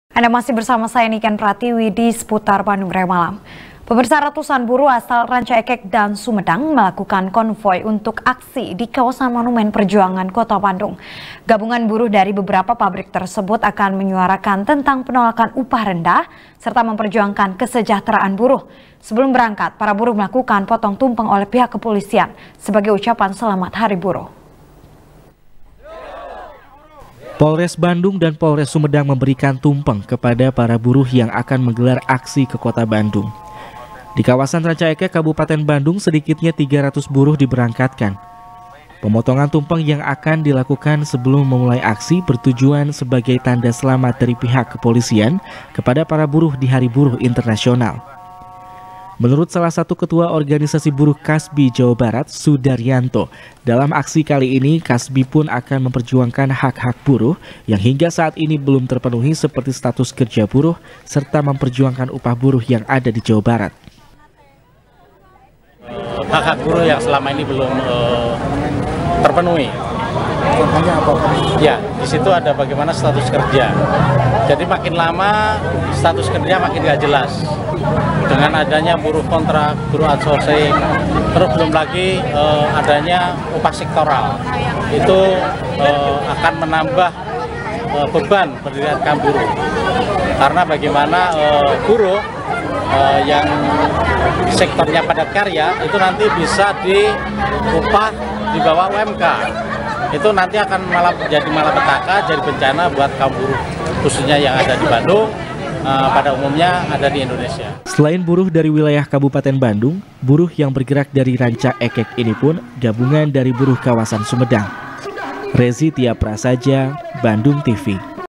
Ratusan Buruh KAB.Bandung Dan Sumedang Bergerak Menuju Bandung | SBR | BANDUNGTV
SBR | BANDUNGTV | NIKEN PRATIWI | Ratusan buruh asal Rancaekek dan Sumedang, melakukan konvoi untuk aksi di kawasan Monumen Perjuangan, Kota Bandung. Gabungan buruh dari beberapa pabrik tersebut, akan menyuarakan tentang penolakan upah rendah, serta memperjuangkan kesejahteraan buruh. Sebelum berangkat, para buruh melakukan potong tumpeng oleh pihak kepolisian, sebagai ucapan selamat Hari Buruh.